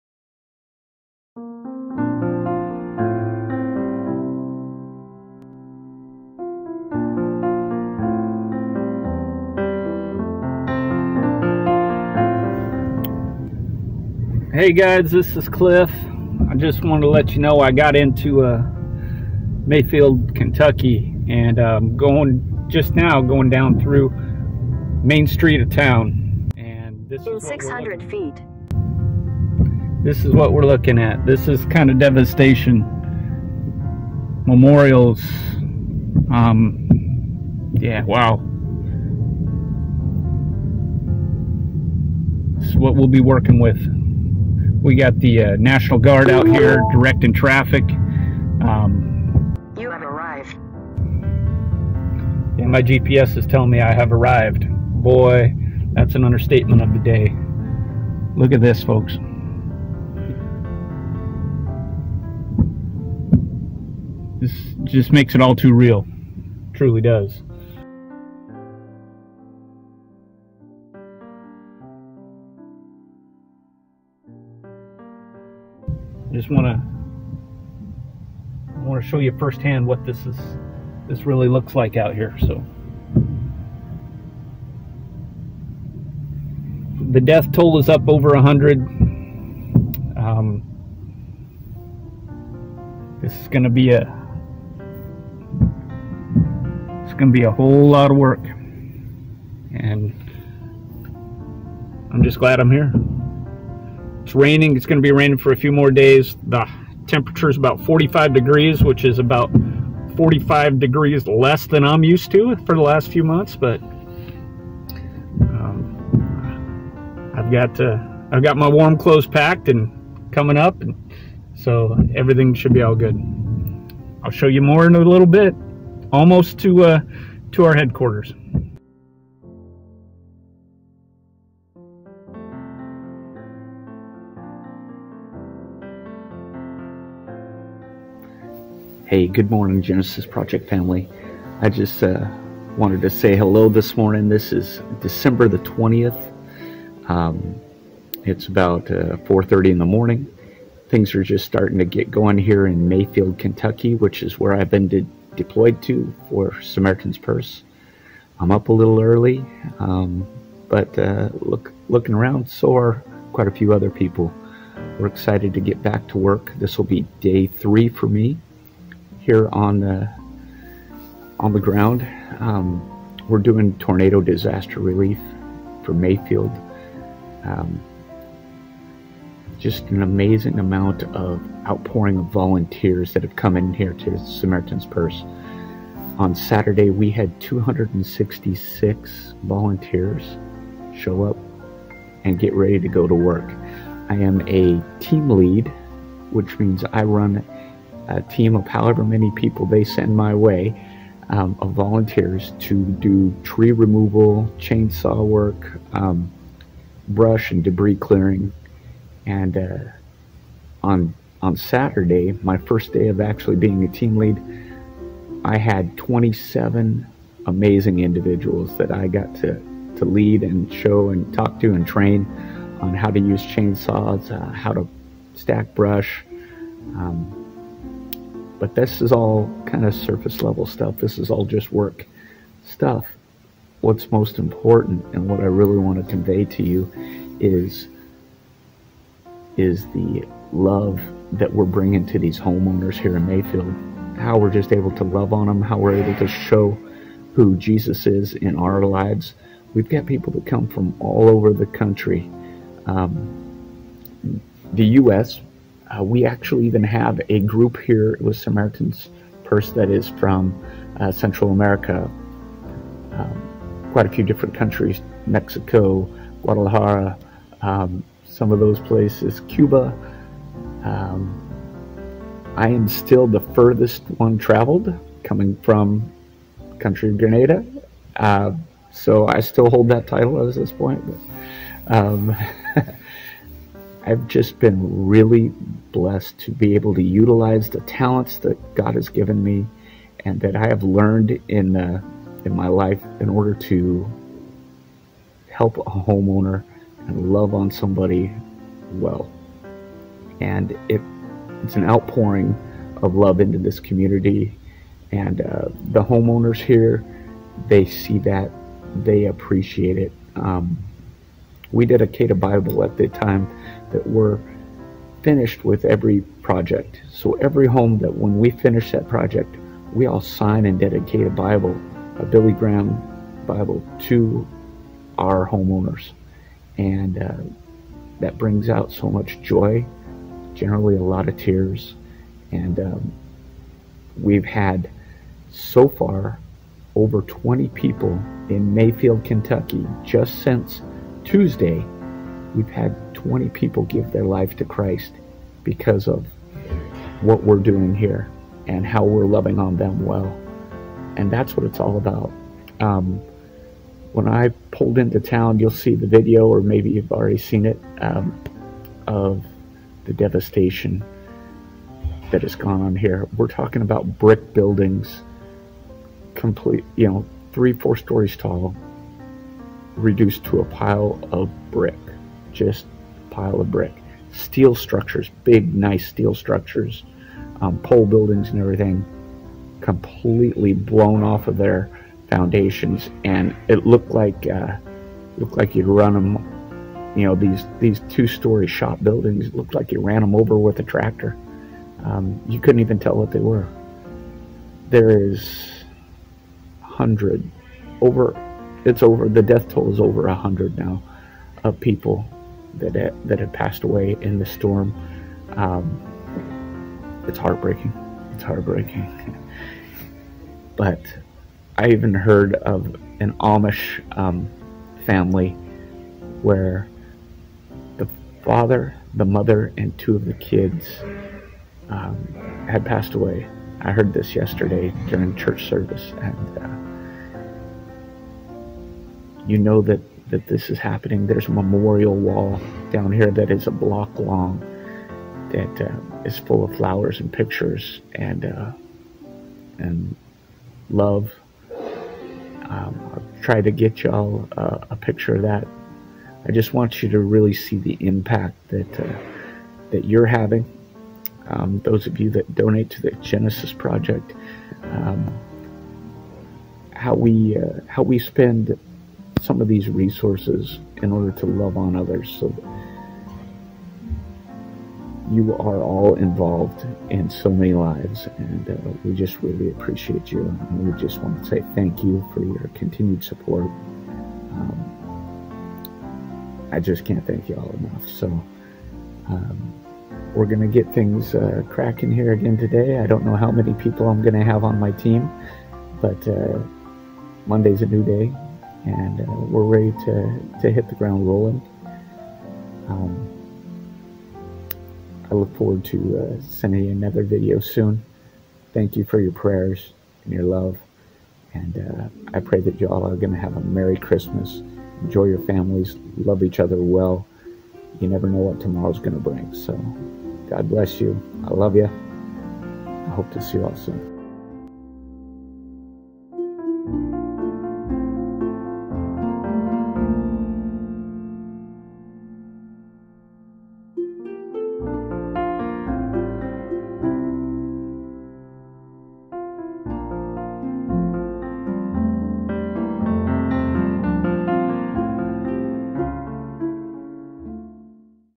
The Genesis Project- Kentucky Tornado Relief Efforts
Why Cliff and I choose to spend Christmas apart this year? The answer was simply a no brainer for us. After hearing and seeing pictures of the devastation and lives lost in Kentucky both online and via media outlets Cliff knew he needed to be there. We are called to serve and this is the best way we know how to serve, by using our gifts, finances and talents. Follow us to get updates of how God is using men and women to touch and change lives in Mayfield Kentucky after the recent tornado.